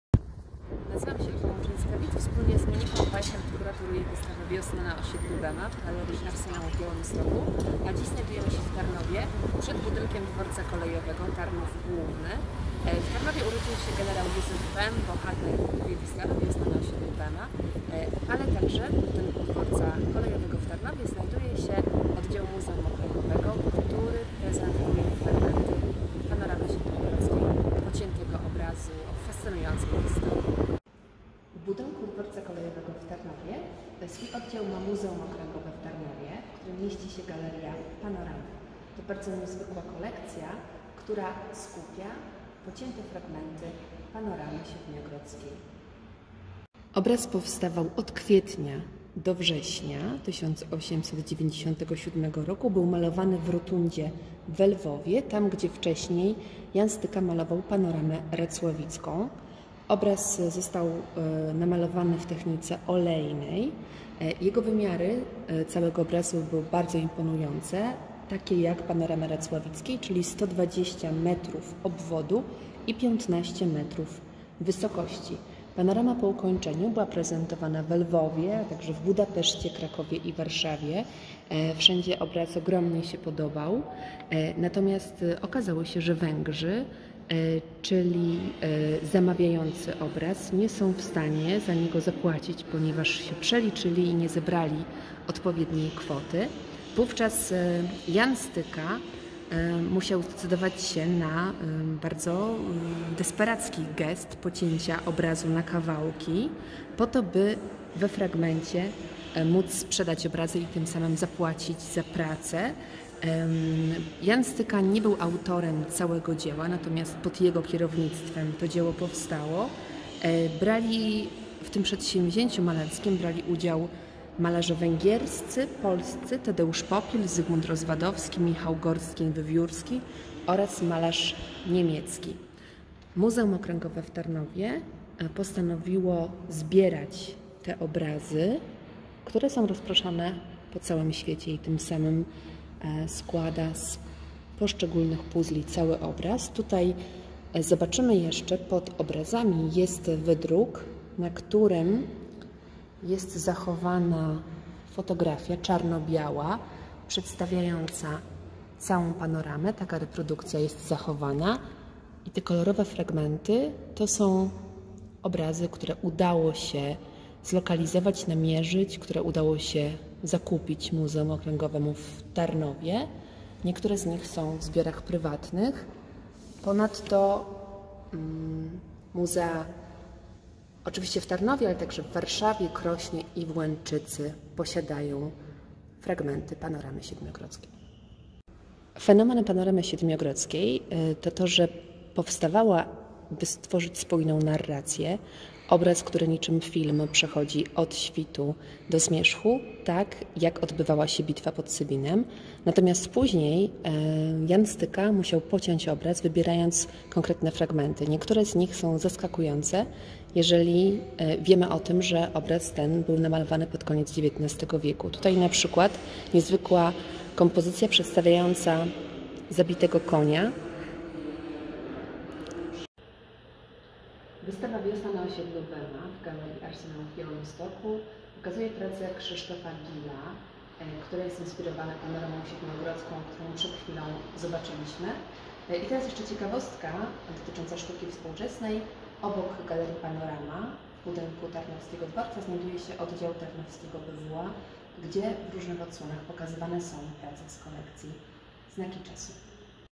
"Wiosna na osiedlu Bema" | "Panorama Siedmiogrodzka" Muzeum Okręgowe w Tarnowie
Ewa Łączyńska-Widz oprowadza po Galerii Panorama – oddziale Muzeum Okręgowego w Tarnowie, która prezentuje fragmenty Panoramy Siedmiogrodzkiej. Te obrazy były inspiracją dla dzieła Krzysztofa Gila.
Wiosna na osiedlu Bema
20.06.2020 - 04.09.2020
Galeria Arsenał, ul. A. Mickiewicza 2

Artyści: Teodor Ajder, Paweł Baśnik, Przemek Branas, Wiktoria Czechowska-Antoniewska , Krzysztof Gil, Andrzej Heidrich, Pravdoliub Ivanov, Martyna Jastrzębska, Nikolay Karabinovych, Szczepan Kopyt, Konrad Kuzyszyn, Diana Lelonek, Dorota Podlaska, Daniel Rumiancew, Konrad Smoleński; projekt na działkach: Agnieszka Dragon i Agnieszka Dybowska.
Kuratorki: Ewa Łączyńska-Widz, Monika Weychert
Projekt wystawy: Matosek/Niezgoda

Montaż zdjęć: Miłosz Onak; materiały dotyczące powtórnego pochówku gen. Józefa Bema dzięki uprzejmości Muzeum Okręgowego w Tarnowie

„Wiosna na osiedlu Bema” to wystawa o polityce historycznej i nekroprzemocy. Zarówno tej dotyczącej ciał: odgrzebywanych, transportowanych, pokazywanych, jak w wypadku szczątków gen. Józefa Bema – jak też nekroprzemocy symbolicznej. Martwe ciała mają dla polityków tę zaletę jako symbole, że nie są już w stanie za siebie przemówić (choć kiedyś mogły), można zatem przypisać im dowolne słowa i idee.
Tytuł wystawy nawiązuje do jednego z białostockich osiedli skupionego wokół ulicy generała Józefa Bema, której nazwa została nadana równo 90 lat temu.